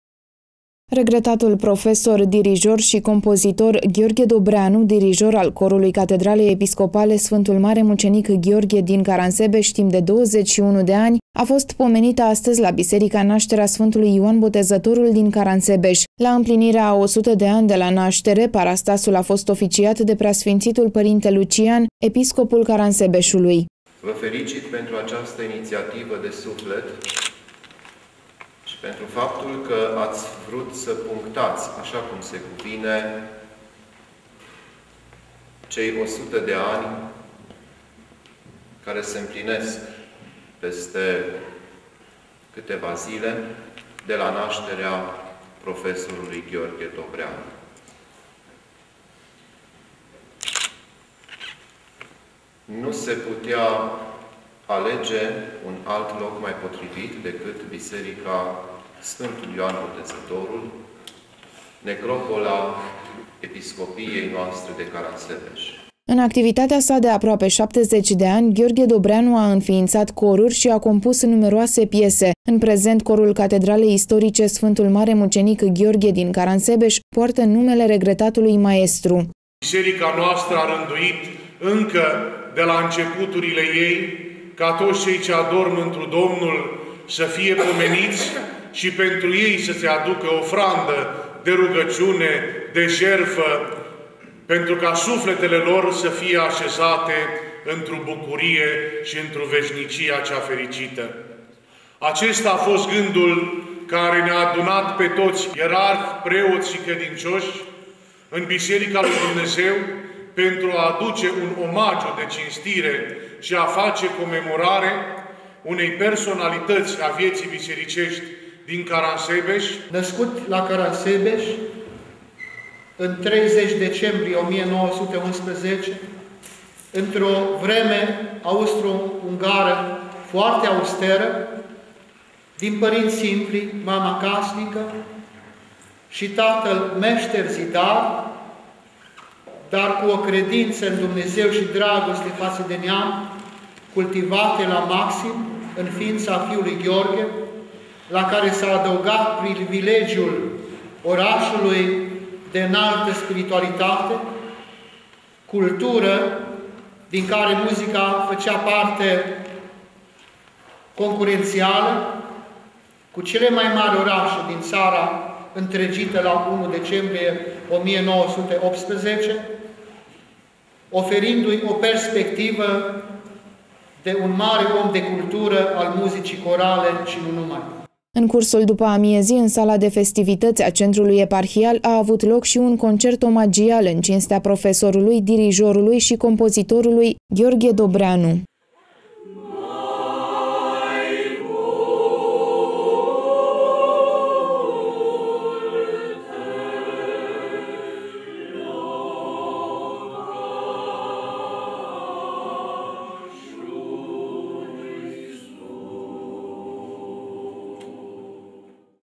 Omagiere şi recunoştinţă la Caransebeş
Zi de omagiere şi recunoştinţă în memoria regretatului profesor, dirijor şi compozitor Gheorghe Dobreanu